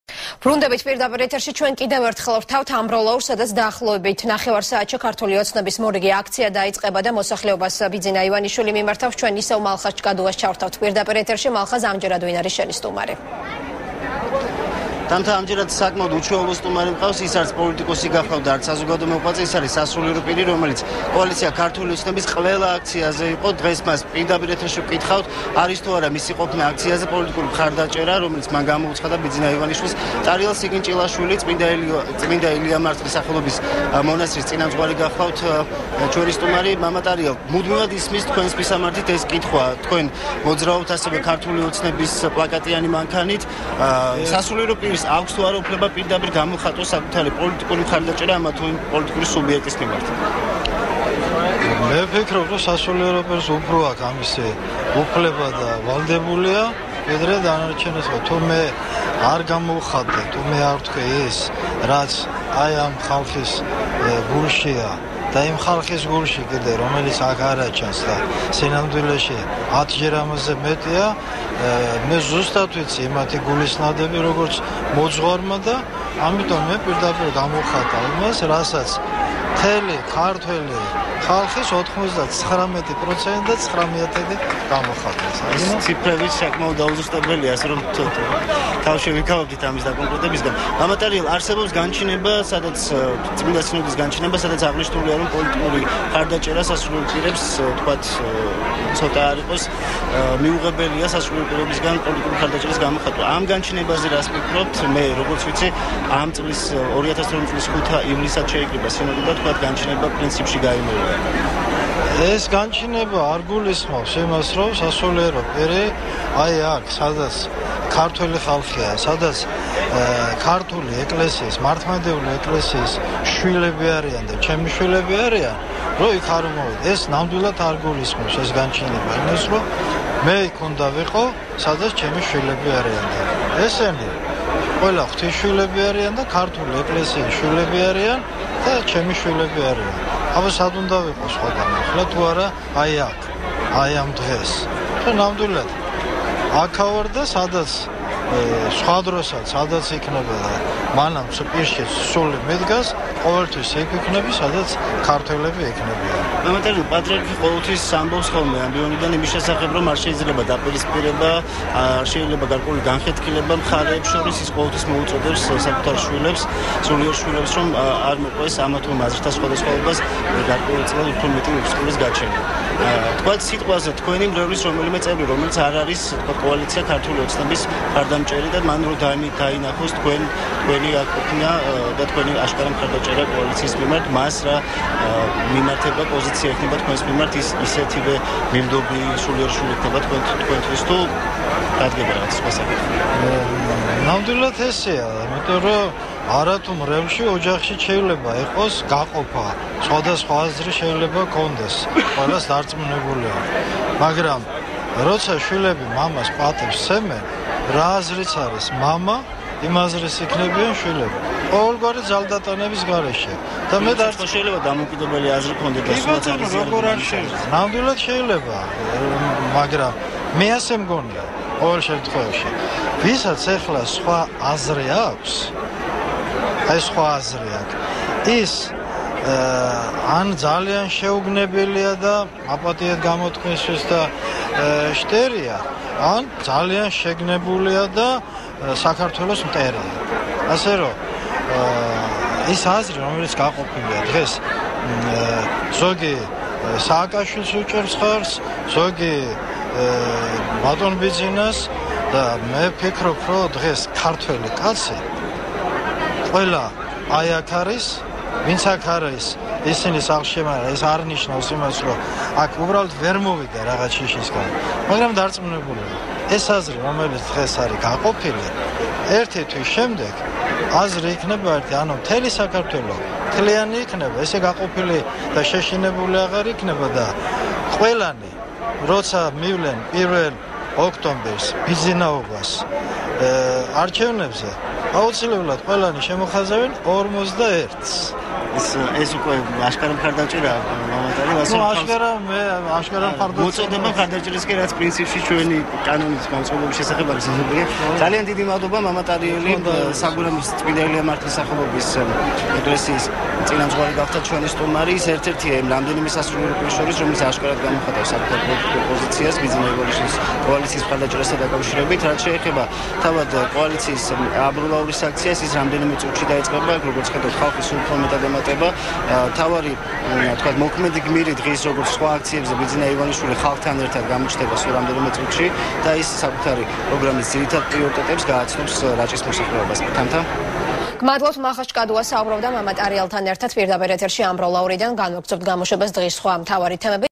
ახალი 6 | ინტერვიუ მამა ტარიელთან | 27.08.12
პირდაპირი ჩართვა ამბროლაურიდან.